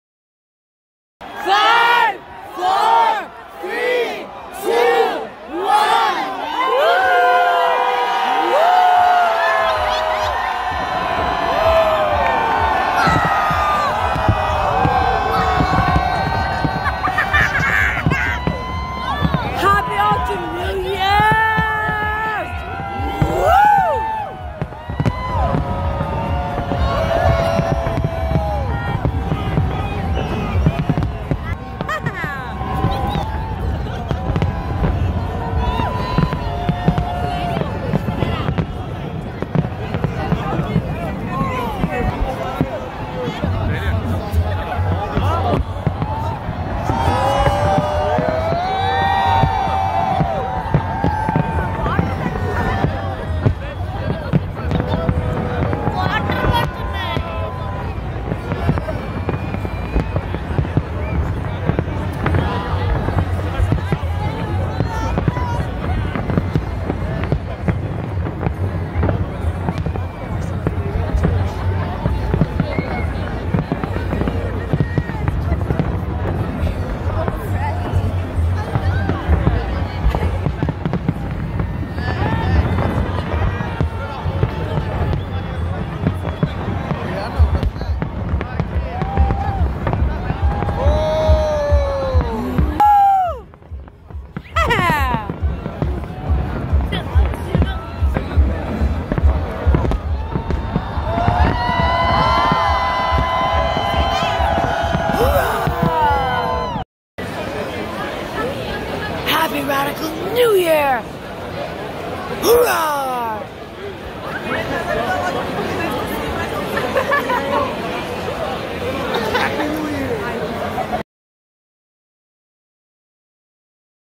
Happy New year 2024 Becca Cala Roarr!
Happy / hapPpy new years yoL!

here please see the racial party that happened at the toronto harbour!

~ Roarr!